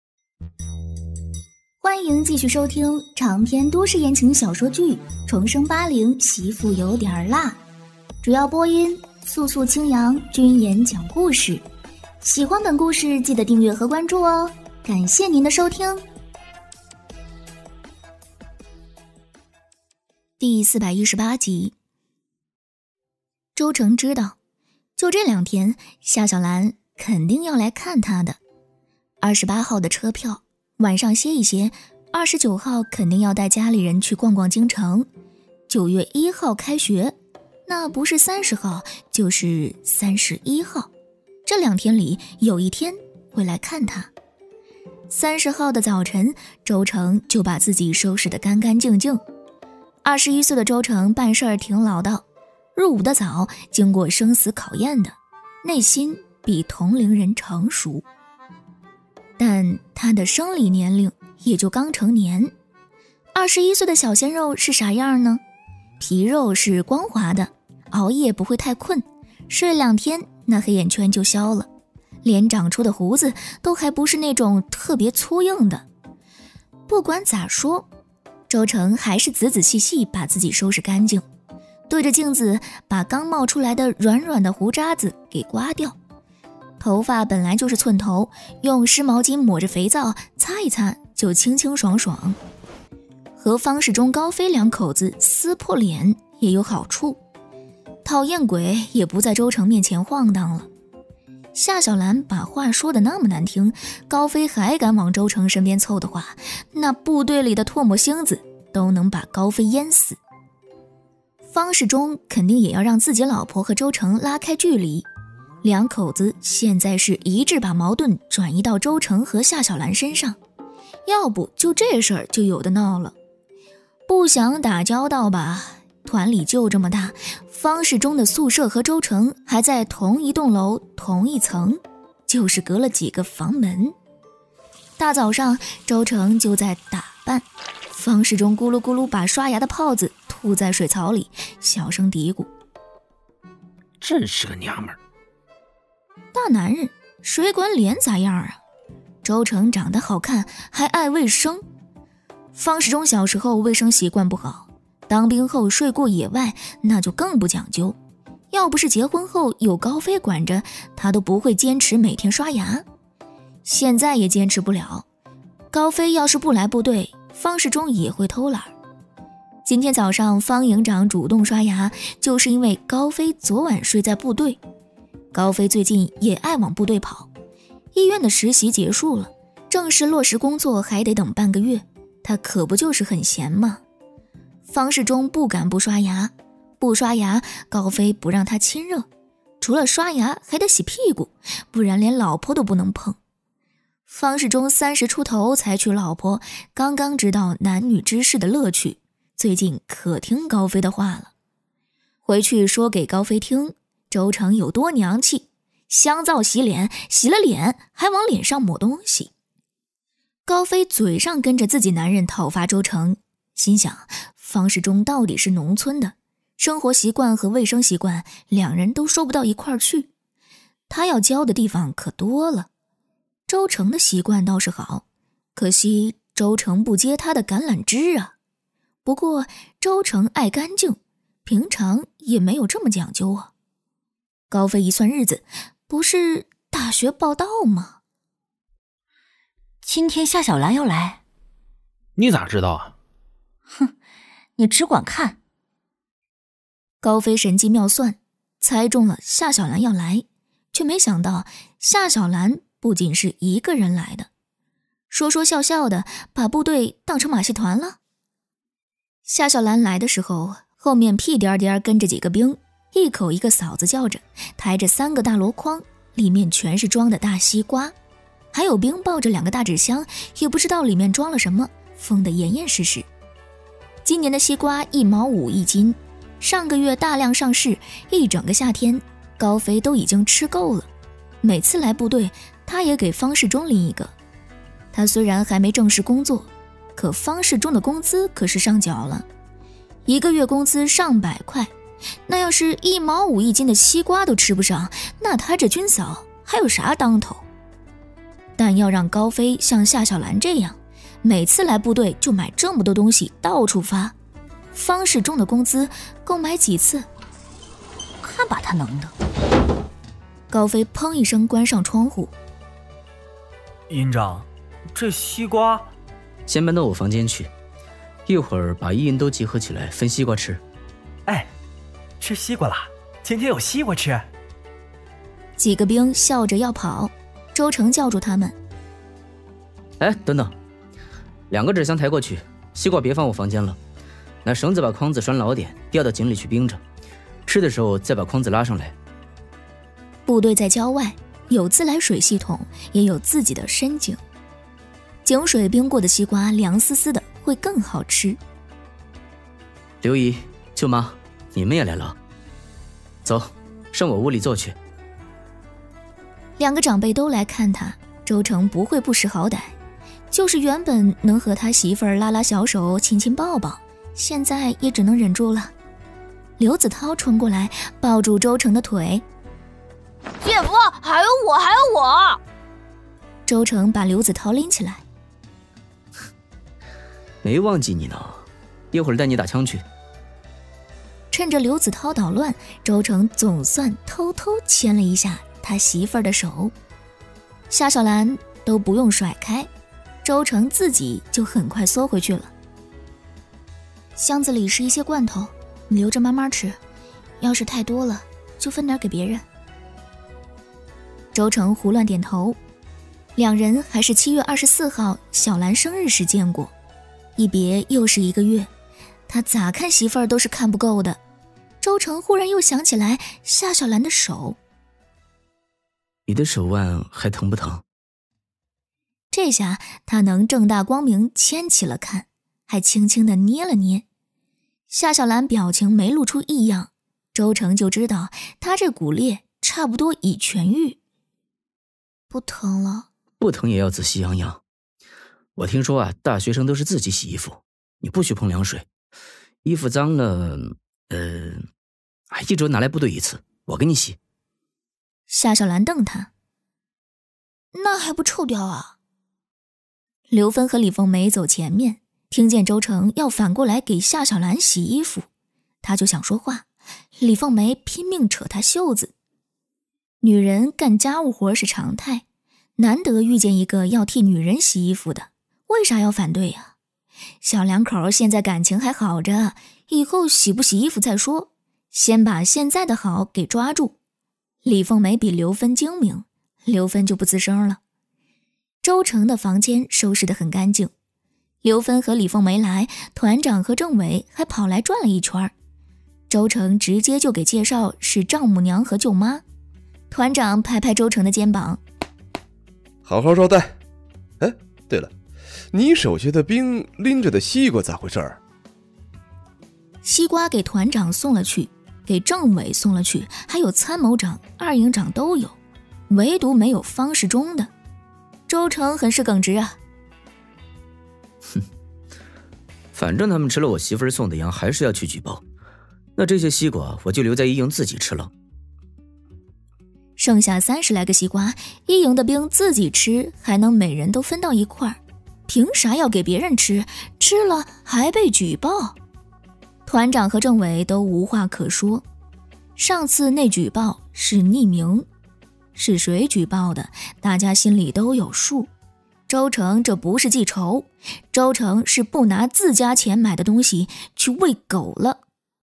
重生八零 媳婦有點辣418
父母早逝，家境貧寒，姿色平平，夏曉蘭抓了一手爛牌，奮鬥了小20年，她當上跨國公司高管，終於將人生的逆境理順了……一覺醒來發現自己重生到了80年代，也叫夏曉蘭，還長了一張（禍）國殃民的臉。同名同姓的“夏曉蘭”拿了一副好牌，卻在流言逼迫下選擇了自殺。手腕強，性格辣的夏曉蘭接過這爛攤子，踩極品，虐渣渣，牽手那對她一見鍾情的男人.